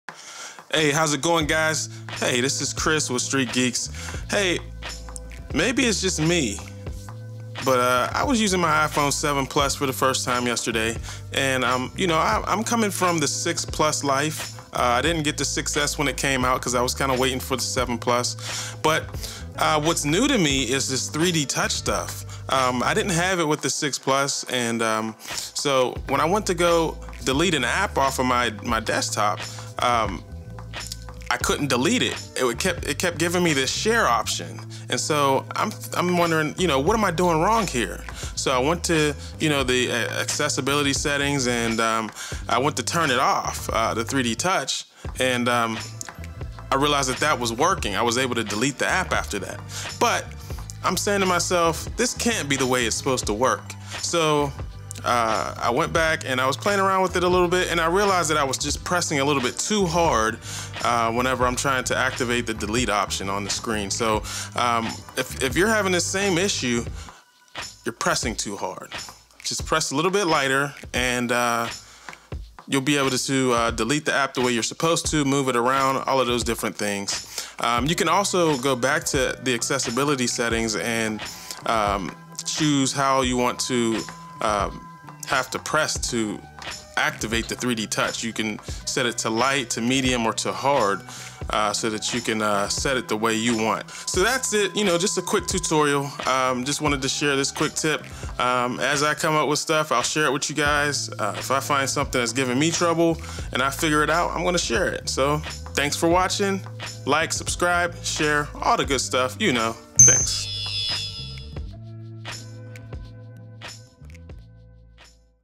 How To Delete/Move Apps on iPhone 7
Quick tip on how to delete/move apps on the new iPhone 7.